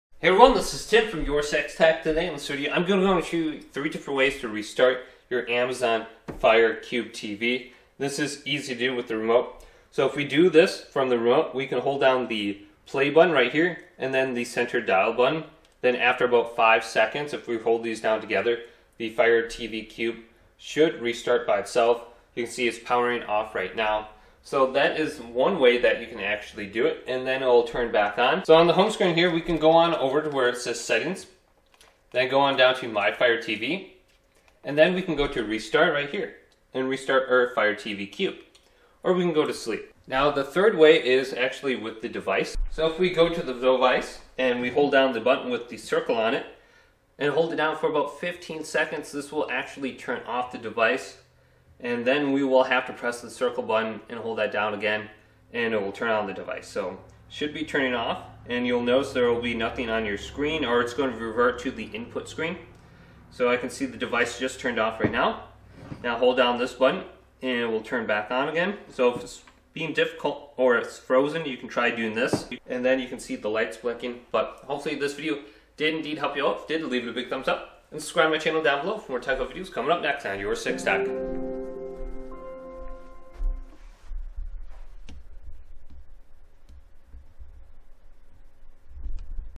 How to Restart Fire TV Cube (3 Different Ways!)
To restart your Fire TV Cube, you can follow these steps:

Using the Remote:

Pick up the Fire TV Cube remote.
Press and hold the Play/Pause button and the Select (center) button at the same time for about 5-7 seconds.
A message will appear on the screen asking if you want to restart your device.
Select "Restart" or "OK."
Using the Settings Menu:

Navigate to the home screen on your Fire TV Cube.
Scroll to the top and select "Settings."
Scroll to the right and choose "Device."
Select "Restart" or "System" (the wording may vary depending on your Fire TV Cube model).
Confirm the restart when prompted.
Using Voice Command (Alexa):

Simply say, "Alexa, restart Fire TV" or "Alexa, reboot."